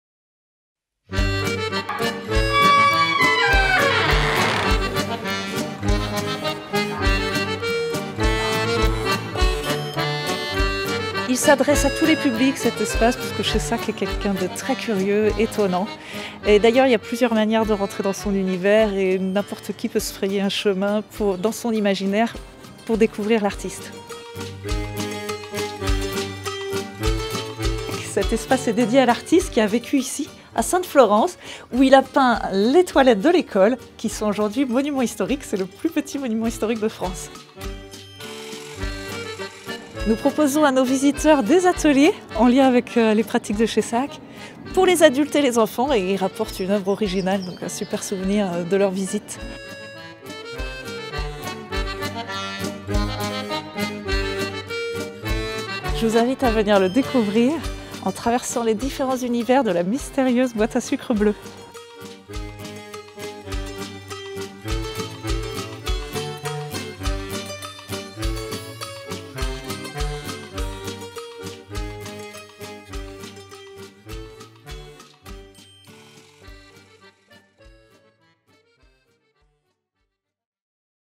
Espace Gaston Chaissac en Vendée - Culturel, insolite et familial
Un espace culturel insolite et artistique accessible à toute la famille, dédié à l'univers insolite de l'artiste inclassable Gaston Chaissac qui a vécu à Sainte Florence dans les années 1950 en Vendée.

Venez découvrir le site classé plus petit Monument Historique de France - La scénographie de la mystérieuse ''boîte à sucre bleue'' - La salle des ressources avec les ateliers pédagogiques et la boutique.

Réservation conseillée. Retrouvez toutes les infos pratiques, horaires et tarifs sur le site Web
Espace Gaston Chaissac - 1 rue de la Scierie - 85140 SAINTE FLORENCE